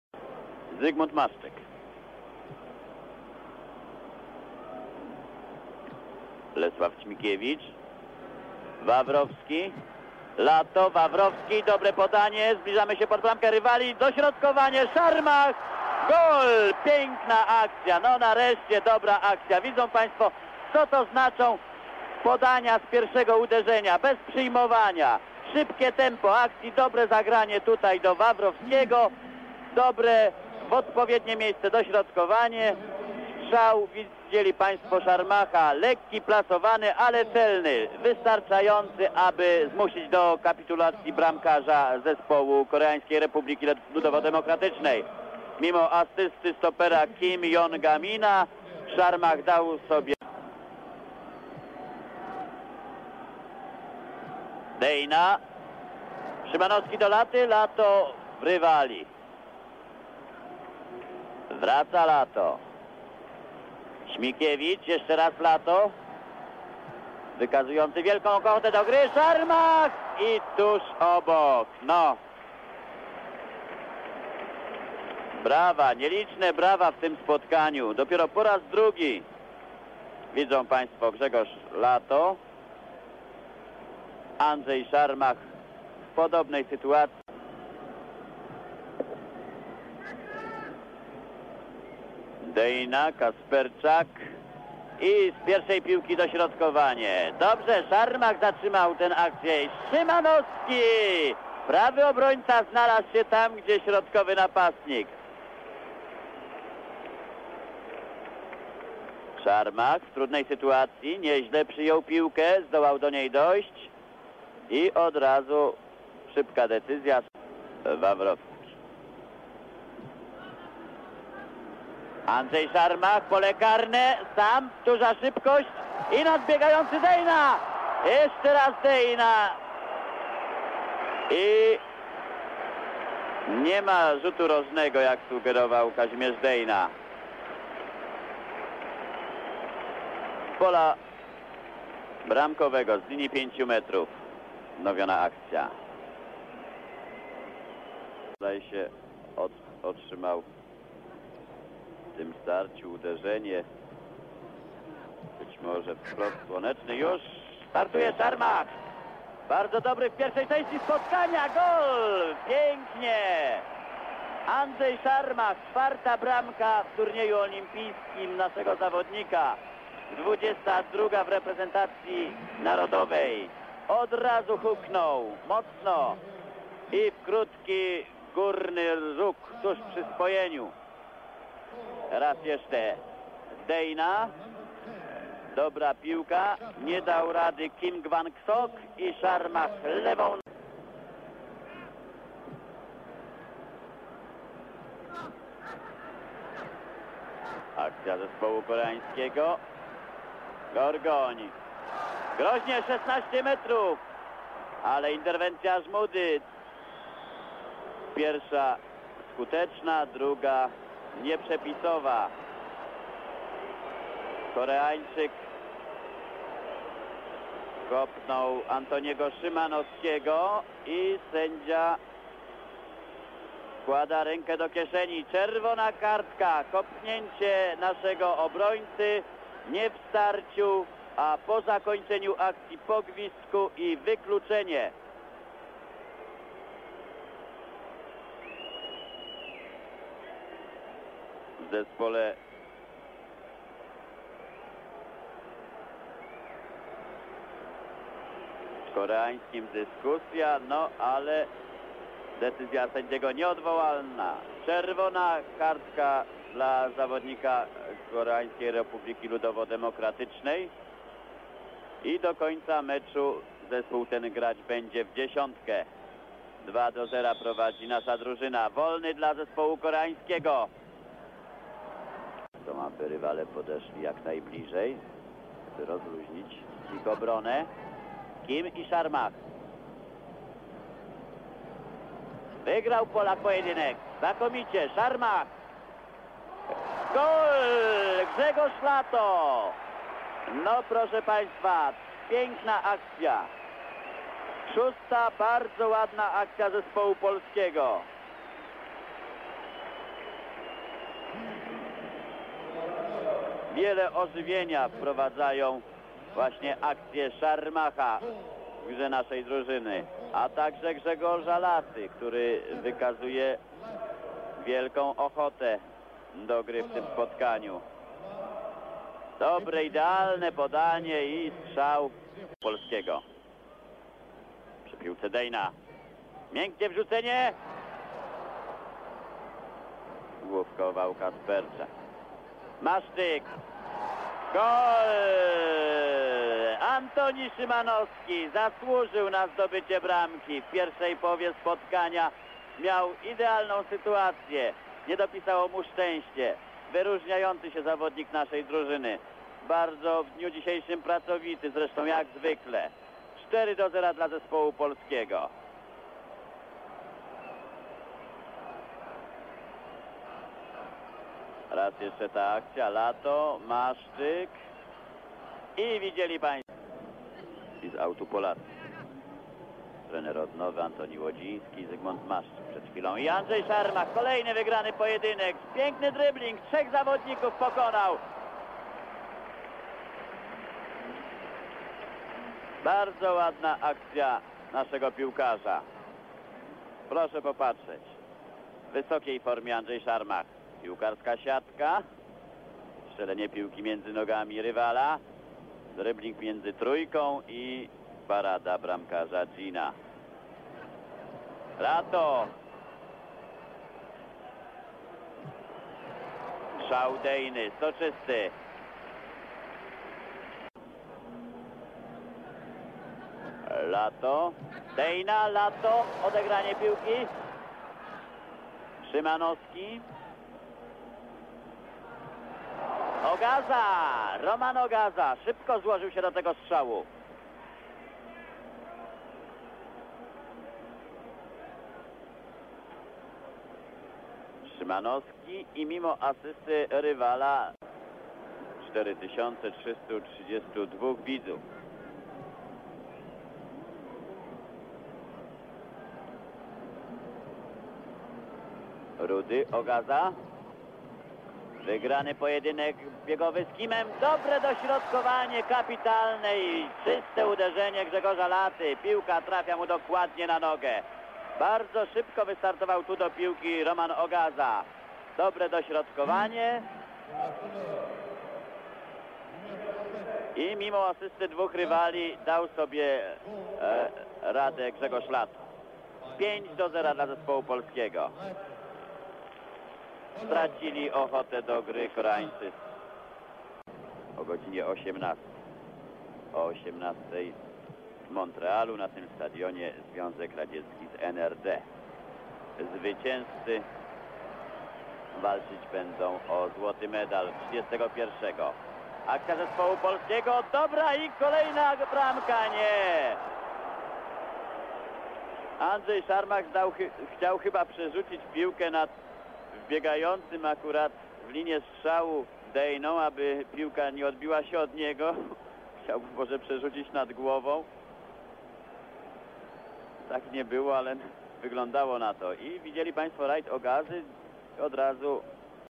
[304] Polska v Korea Płn [25/07/1976] Poland v North Korea
1976 [304] Polska v Korea Płn [5-0] Poland v North Korea
Letnie Igrzyska Olimpijskie - Montreal 1976, 25 lipca 1976, Montreal, Olympic Stadium, widzów: 46 855, Sędzia: Paul Schiller (Austria)

Bramki: 1-0 Andrzej Szarmach 14', 2-0 Andrzej Szarmach 49', 3-0 Grzegorz Lato 58', 4-0 Antoni Szymanowski 65', 5-0 Grzegorz Lato 79'

Polska: Jan Tomaszewski – Antoni Szymanowski, Jerzy Gorgoń, Władysław Żmuda, Henryk Wawrowski (64' Wojciech Rudy), Lesław Ćmikiewicz, Henryk Kasperczak, Zygmunt Maszczyk (71' Roman Ogaza), Grzegorz Lato, Kazimierz Deyna (K), Andrzej Szarmach, Trener: Kazimierz Górski

Korea Północna: Jin In Chol – Kim Jong Min, Cha Jong Sok, Kim Mu Gil (21' Kim Gwangsok), Kim Il Nam, An Se Uk [58' cz. k.], Lee Hi Yon, Pak Jong Hun, Hong Song Nam, Yang Song Guk, Myong Dongchan (47' Ma Jong-U)